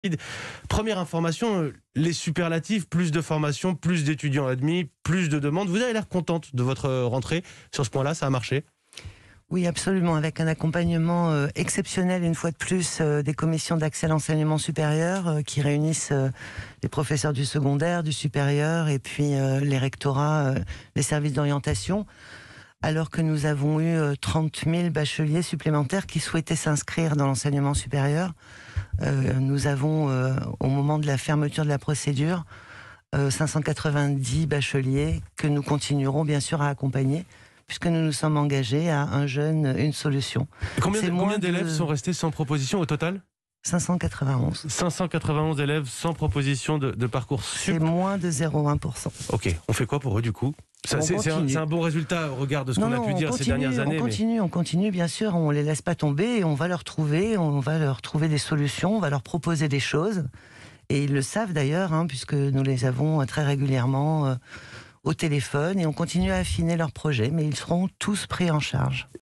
591 bacheliers sont toujours sans proposition sur Parcoursup, annonce Frédérique Vidal
591 bacheliers sont toujours sans proposition sur Parcoursup, annonce Frédérique Vidal